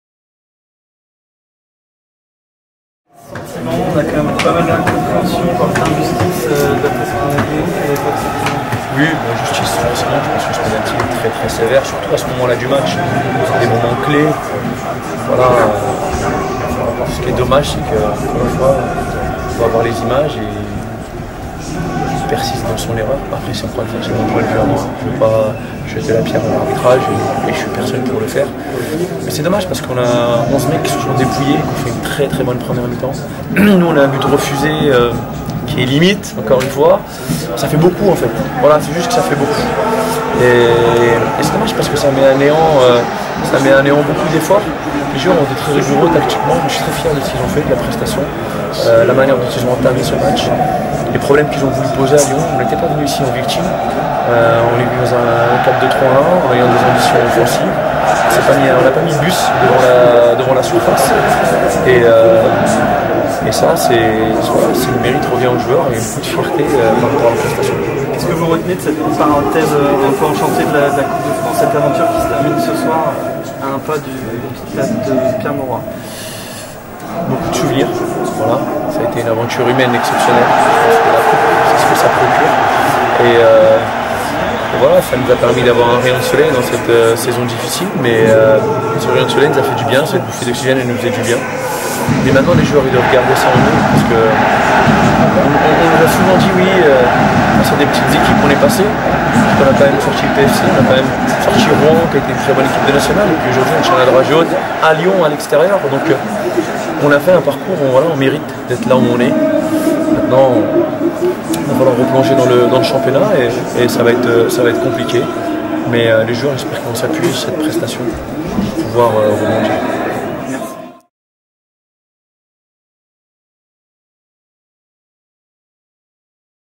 🎙️ Au micro avec Ahmed Kantari après la demi-finale face à Lyon
La réaction du coach après la fin de l'aventure en Coupe de France.